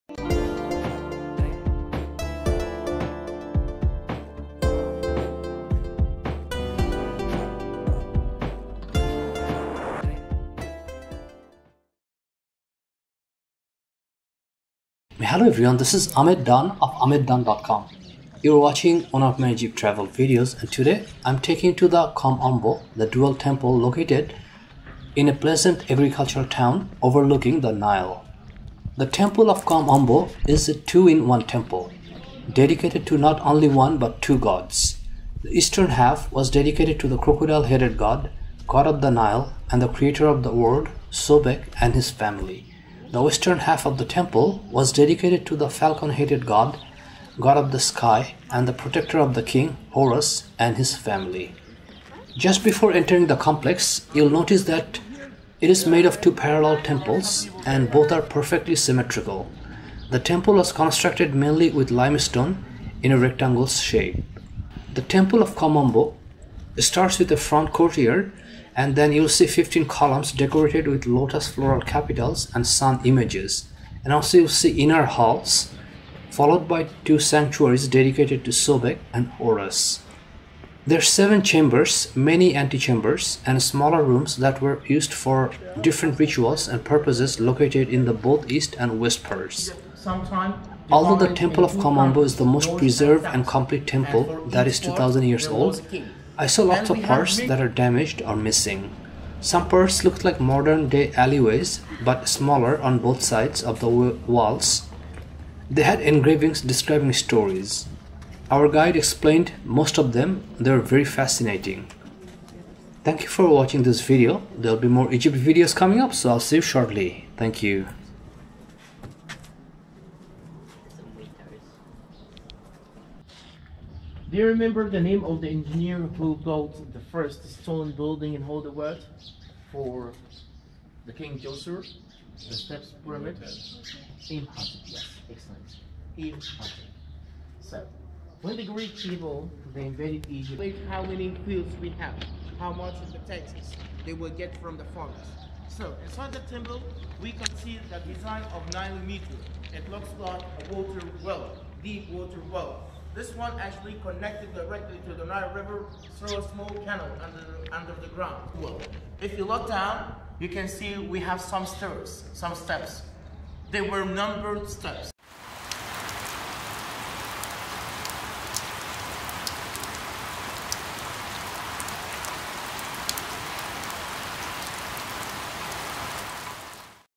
Temple of Kom Ombo | Dedicated to Crocodile God Sobek & Falcon God Horus | Egypt Travel Vlog | 03D19
The Unusual Dual Crocodile & Falcon God Temple of Kom Ombo | Must-See Egypt Travel

The two-in-one temple of Kom Ombo is dedicated to two gods. The eastern half to the crocodile-headed god, god of the Nile and creator of the world, Sobek and his family. The western half to the falcon-headed god, god of the sky and protector of the king, Horus and his family.

The two parallel temples are perfectly symmetrical. The Temple was constructed mainly with limestone in a rectangle shape.

Today, I will take you to the unique dual temple Kom Ombo in Egypt.

Chapters:
0:00 Intro
0:15 Temple of Kom Ombo
0:50 Parallel and Symmetrical Temple
1:05 Inside the Kom Ombo Temple

You may want to watch the following:

Night in The Middle of the Nile In A Felucca Cruise Boat